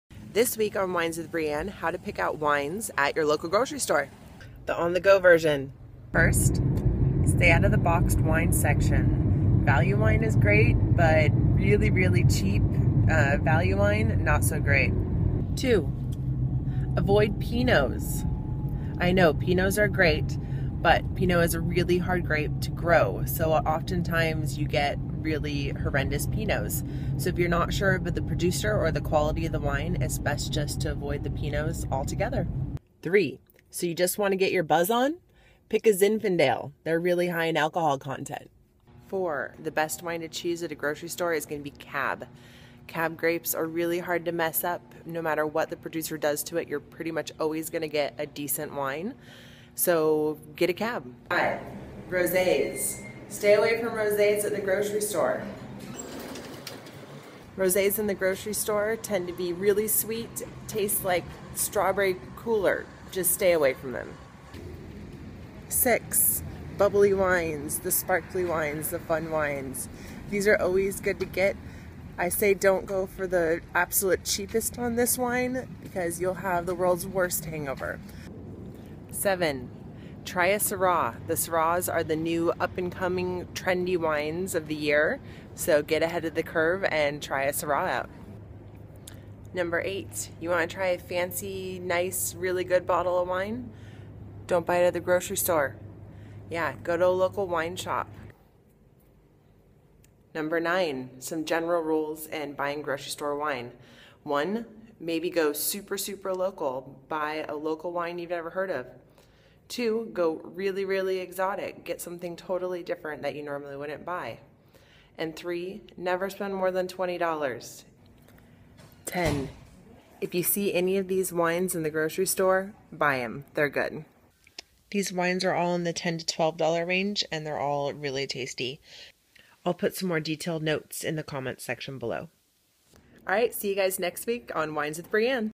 Grocery Store Wine Picks.
Learn how to pick out wines from your local grocery store.